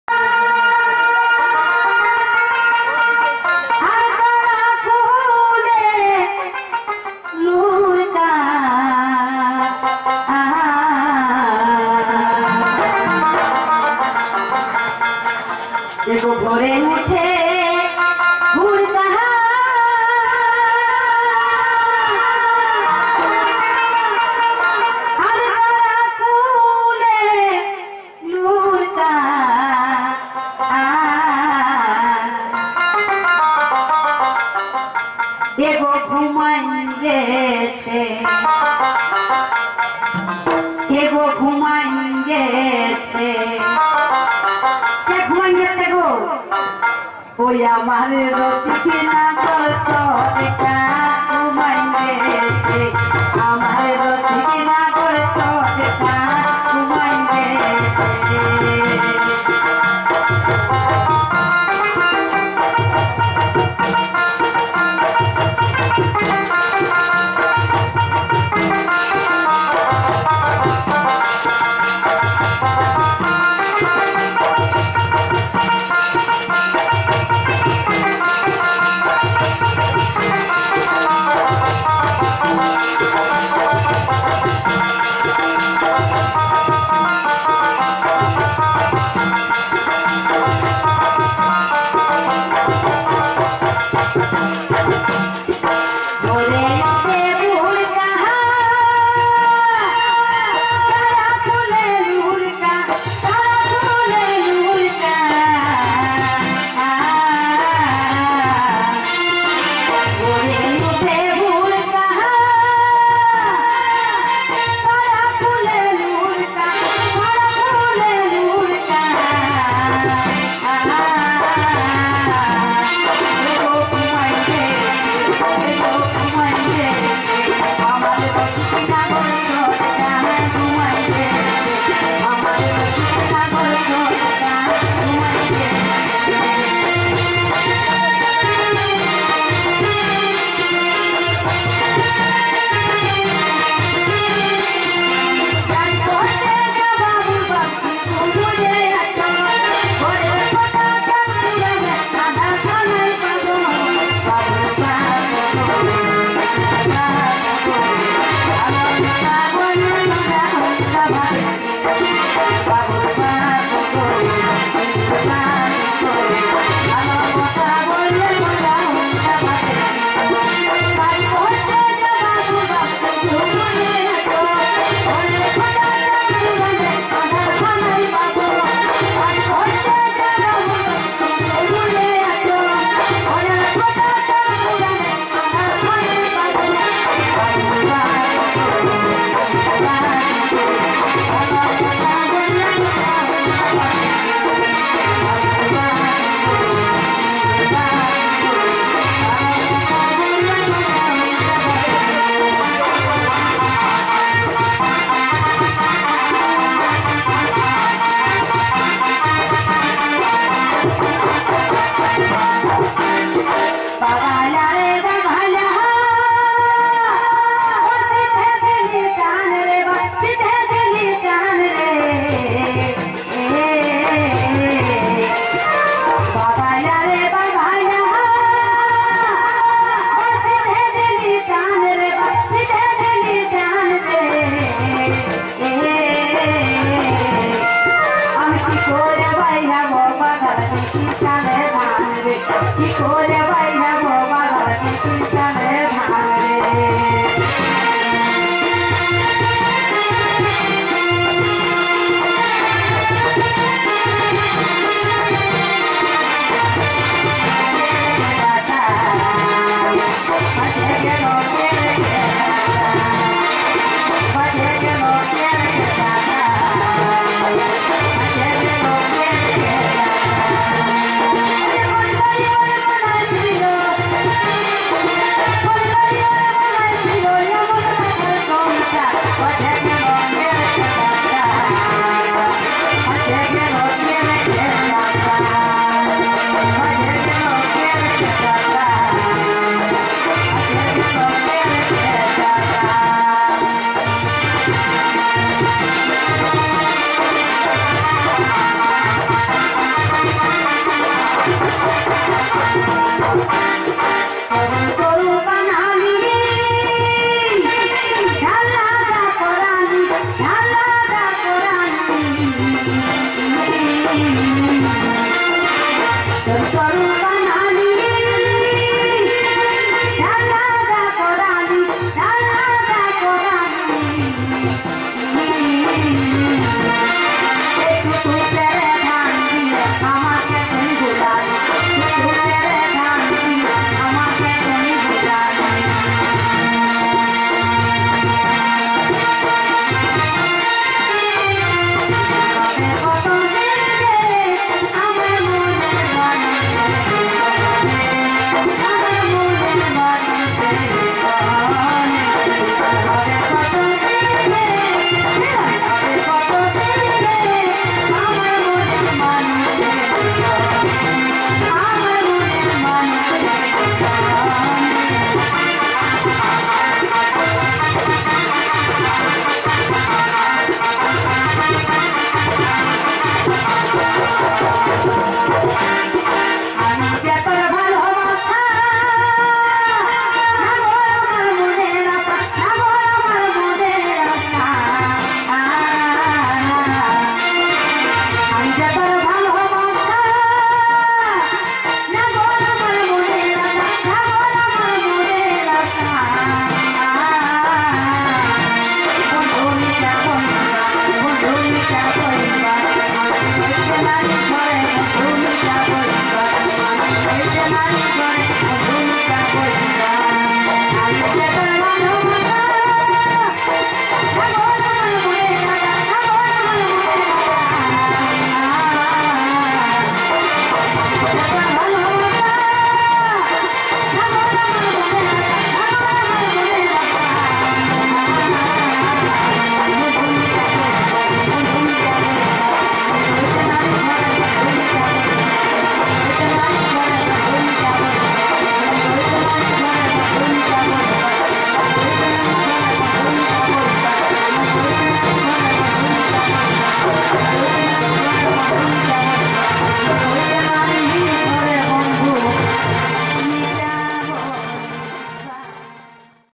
মহিলা ঝুমুর নাচ★Sunita Mahato jhumur Nach◆bongabari◆
Mahila jhumur Nach
Sunita mahato
Bongabari,purulia

Presented by RUPASI PURULIA